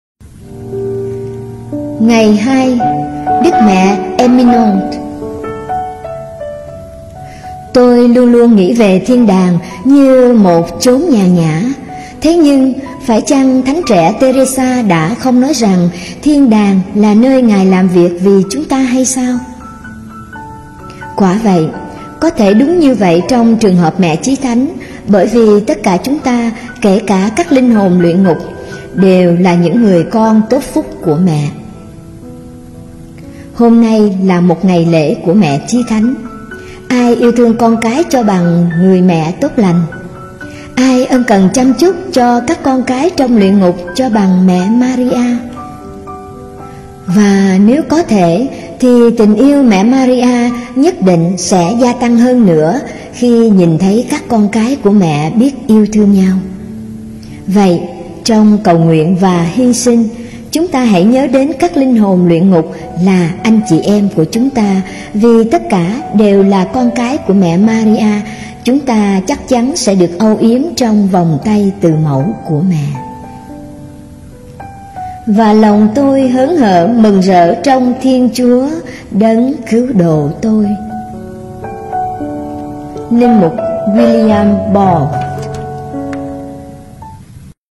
NGÀY NGÀY VỚI MẸ 02.11 - Đức Mẹ Emminont
Đức Mẹ Emminont

Tôi luôn luôn nghĩ về thiên đàng như một chốn nhàn nhã! Thế nhưng, phải chăng thánh trẻ Têrêsa đã không nói rằng thiên đàng là nơi ngài làm việc vì chúng ta hay sao?

Quả vậy, có thể đúng như vậy trong trường hợp Mẹ Chí Thánh, bởi vì tất cả chúng ta – kể cả các linh hồn Luyện Ngục – đều là những người con tốt phúc của Mẹ.

Hôm nay là một ngày lễ của Mẹ Chí Thánh. Ai yêu thương con cái cho bằng người mẹ tốt lành? Ai ân cần chăm chút cho các con cái trong luyện ngục cho bằng Mẹ Maria?

Và nếu có thể, thì tình yêu Mẹ Maria nhất định sẽ gia tăng hơn nữa khi nhìn thấy các con cái của Mẹ biết yêu thương nhau. Vậy, trong cầu nguyện và hy sinh, chúng ta hãy nhớ đến các linh hồn luyện ngục là anh chị em của chúng ta, vì tất cả đều là con cái của Mẹ Maria; chúng ta chắc chắn sẽ được âu yếm trong vòng tay từ mẫu của Mẹ.

… Và lòng tôi hớn hở mừng rỡ trong Thiên Chúa, Đấng Cứu Độ tôi,

Lm. William Boat